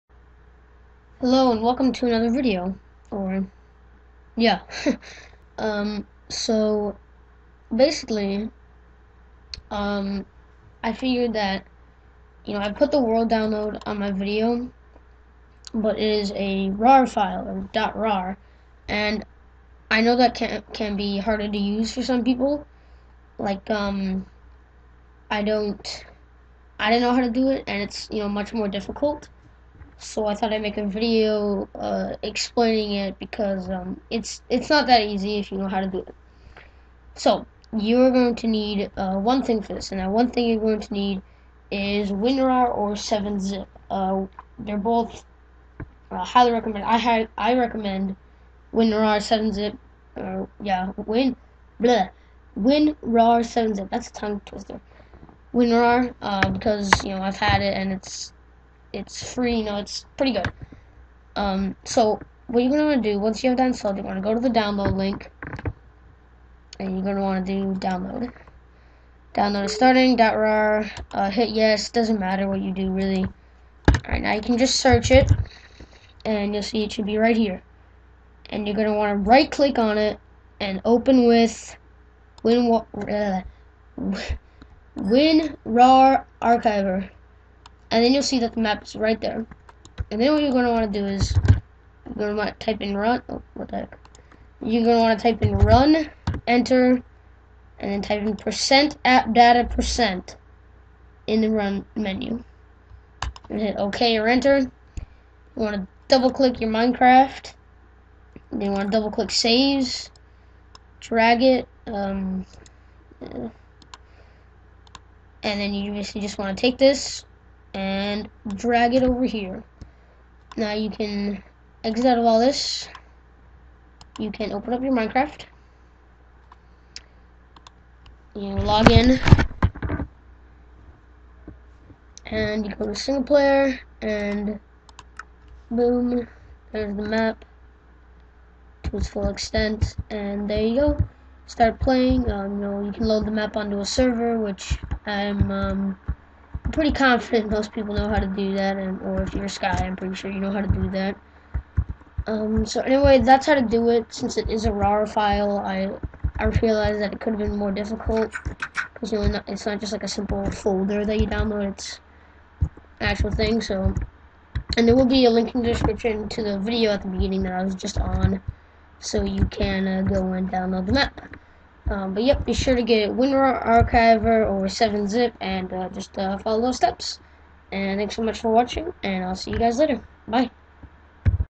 How to install .rar Custom MC Maps!
Tutorial by MinecraftPlayahz showing you how to install maps that are .rar fil